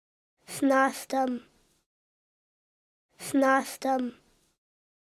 snástem (Saskatoon berry)
Tsitsáyx̱emaat Rebecca Duncan and her students from Xwemélch’stn Etsimxwawtxw Capilano Little Ones visited our Library's Recording Studio to record the Sḵwx̱wú7mesh Sníchim names of the native plants in Swáy̓wi Temíxw, our Community Demonstration Garden.

We are grateful to Tsitsáyx̱emaat and her students for sharing their language and traditional knowledge with us!